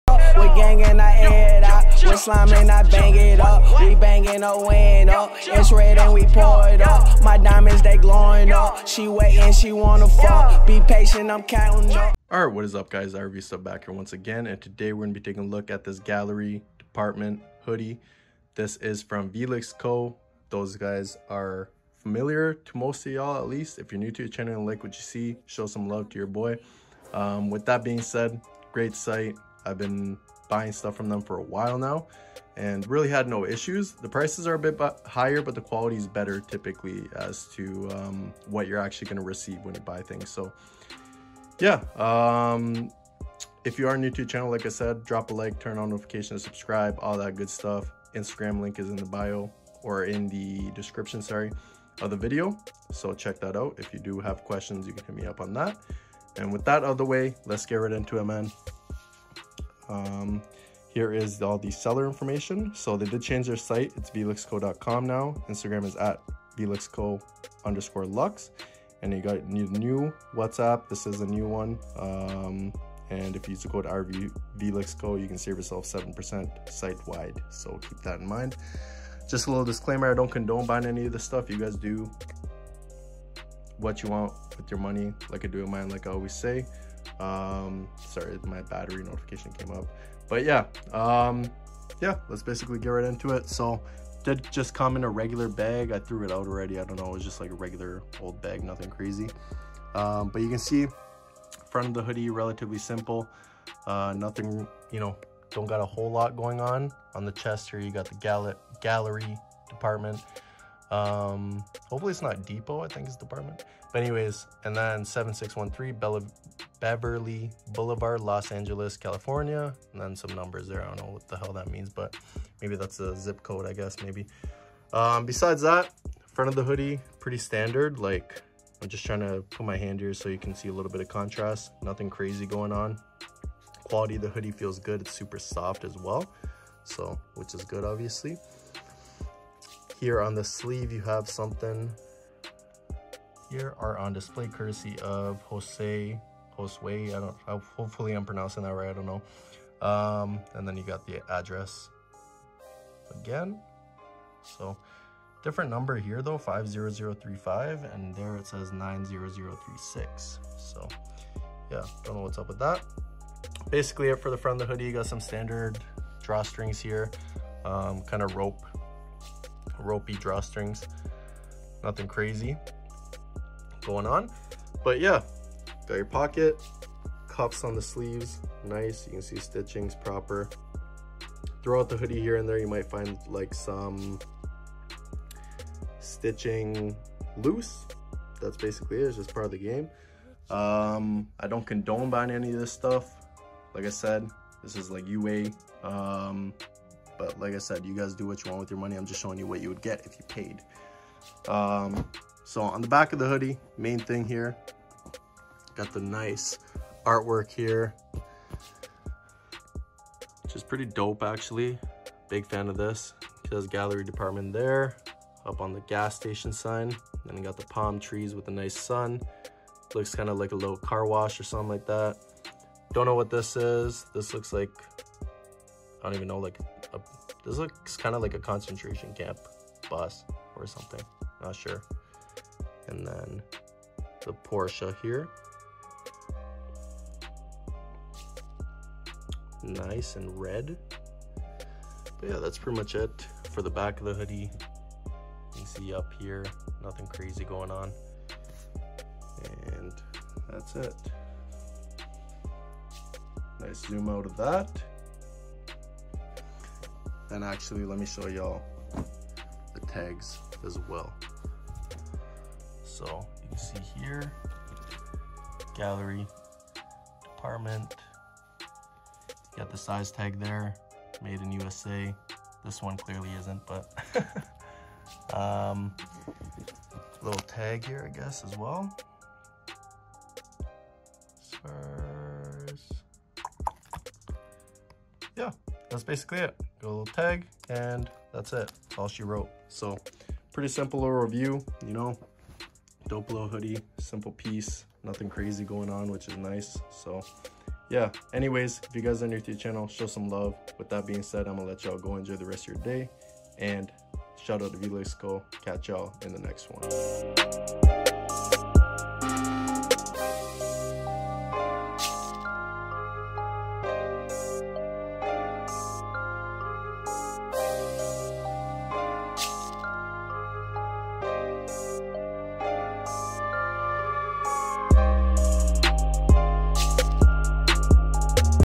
Gallery Dept. Hoodie - Vlixco Review
Hey everybody! In this video I'm reviewing a Gallery Dept. hoodie from Vlixco! Let me know what you think about this hoodie and lets try to hit 50 likes for this video! Thanks! :)